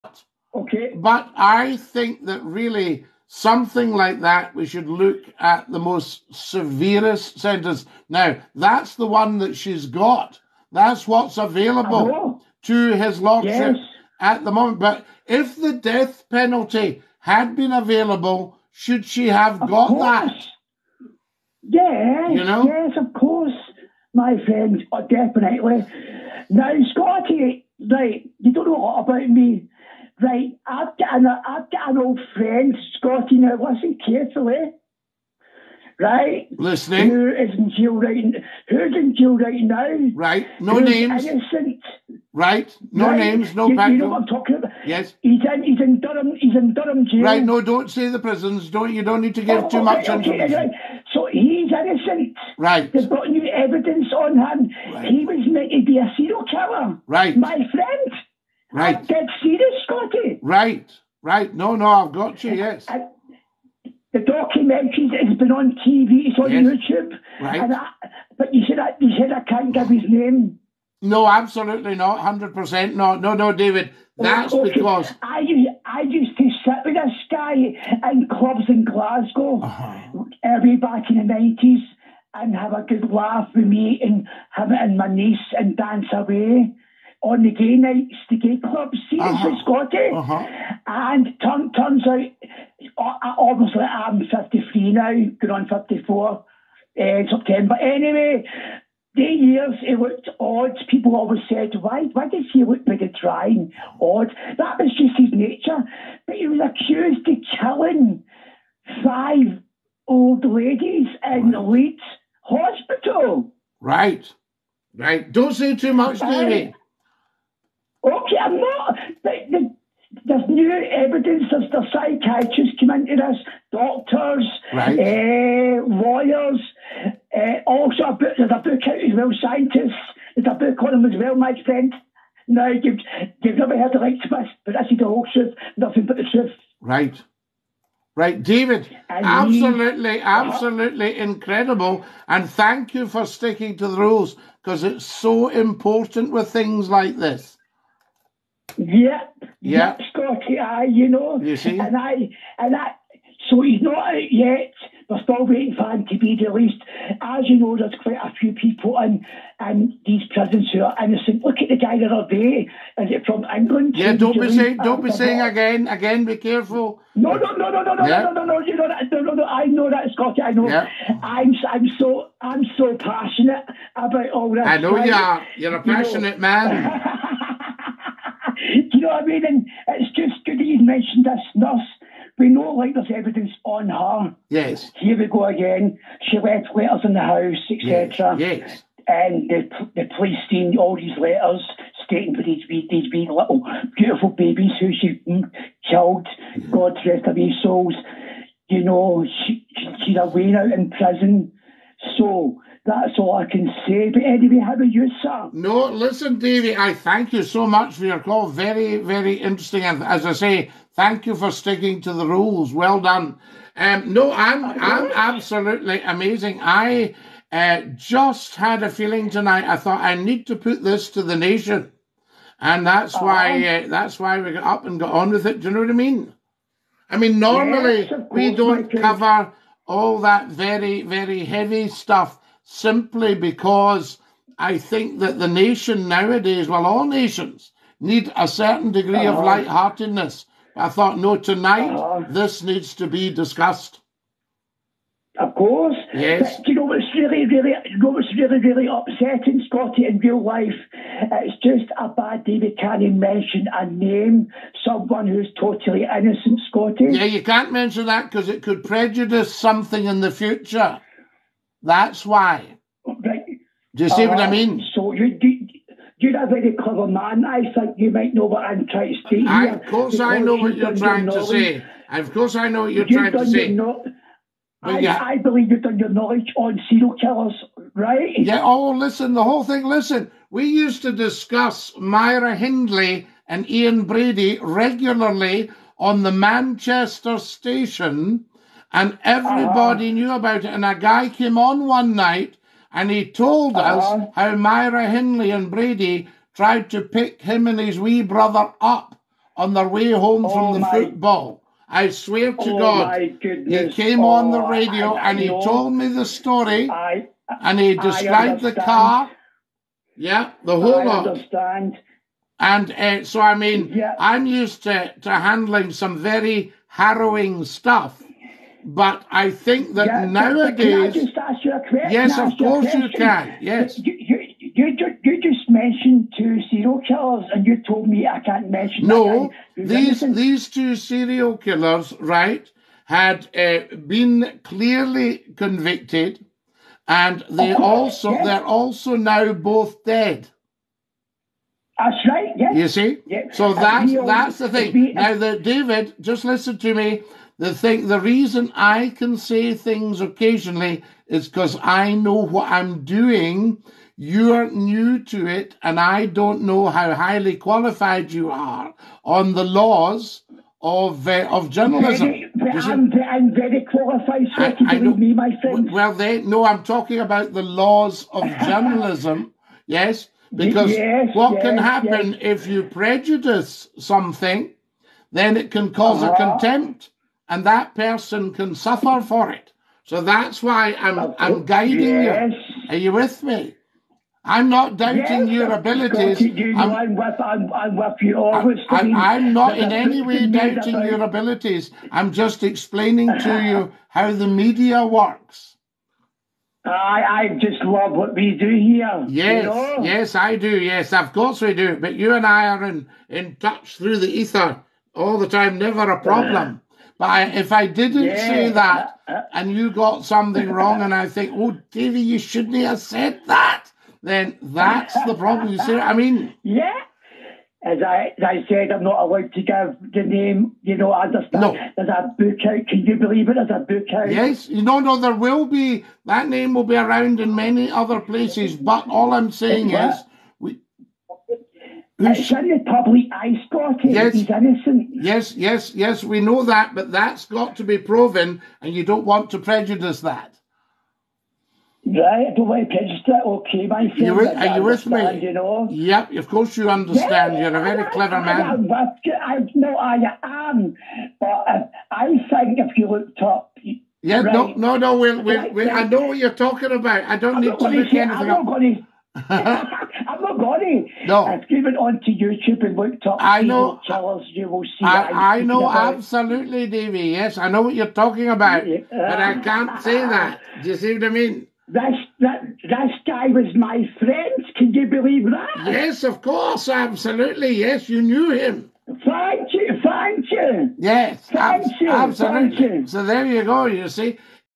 CAPITAL PUNISHMENT LIVE
ABSOLUTELY FASCINATING CALL LIVE TO THE PHONE IN ON THE WORLD'S TOP TALK SHOW
HOSTED BY LEGENDARY BROADCASTER AND INTERNET ICON SCOTTIE McCLUE ON LAW AND ORDER